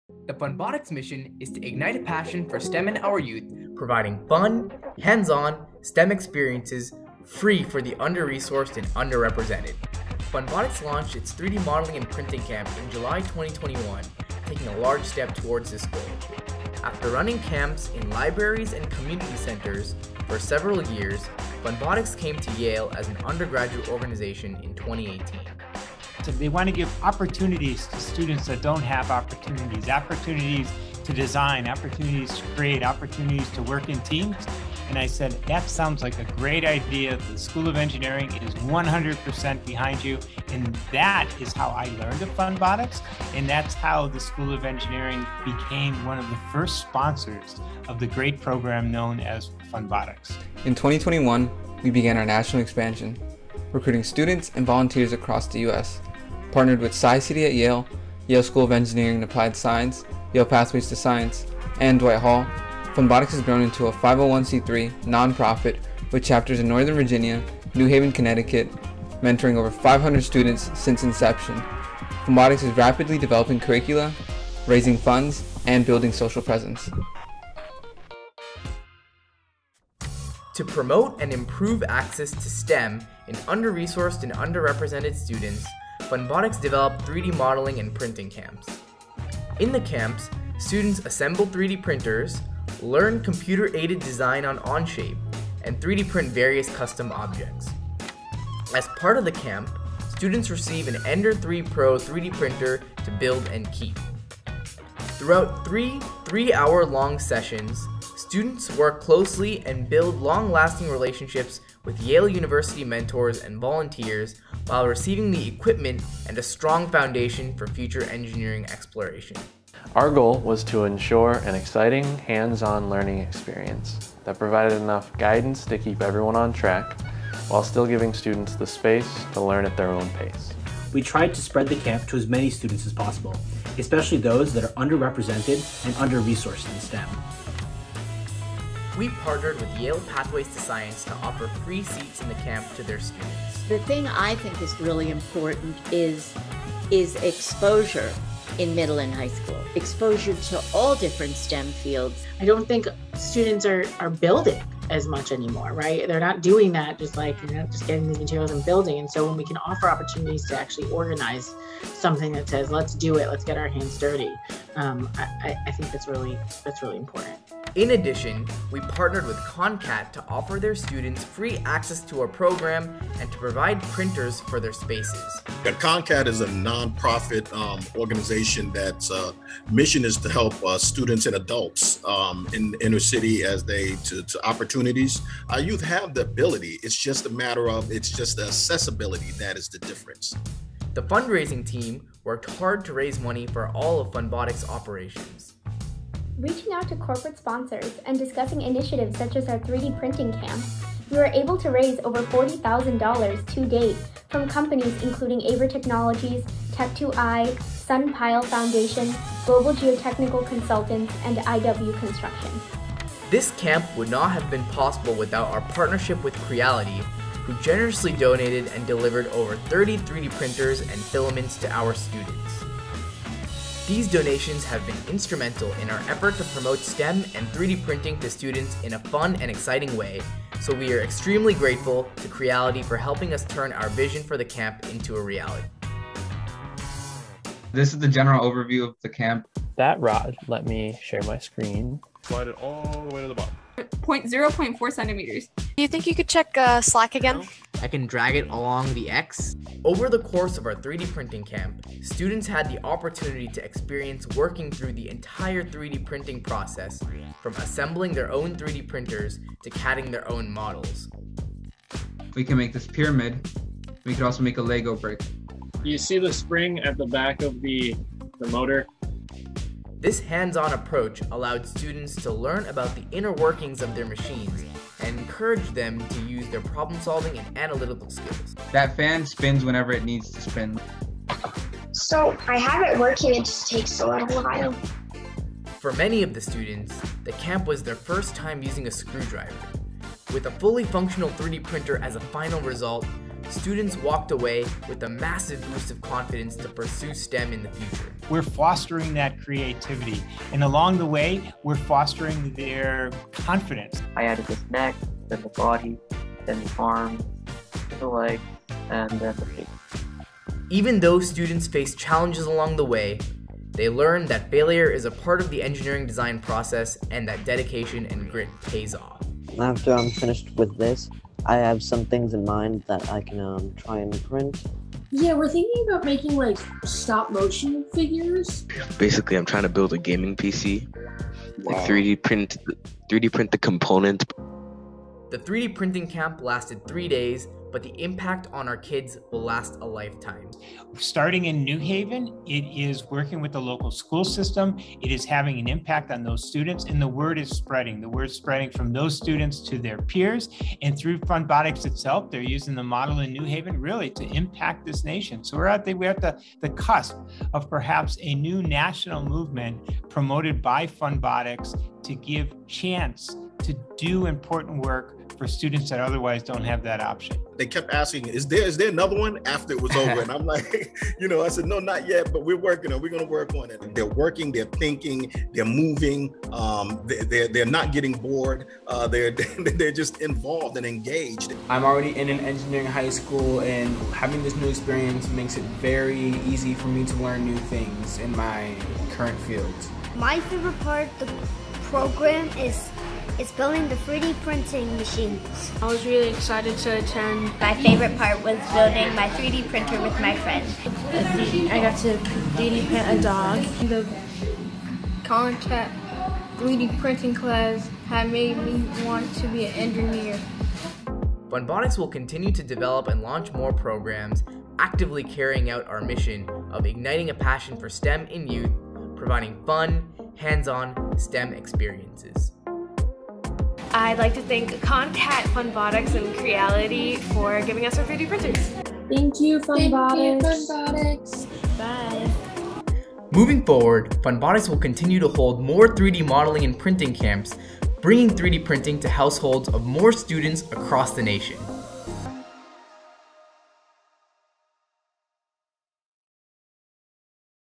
Funbotics 3D Modeling & Printing Camp Documentary
As part of the national expansion of Funbotics, we launched our 3D modeling and printing camp in July 2021! Partnered with Creality, Yale School of Engineering and Applied Science, ConnCAT, and Yale Pathways to Science, the camp had an immeasurable impact on the lives of students, inspiring them to pursue STEM paths.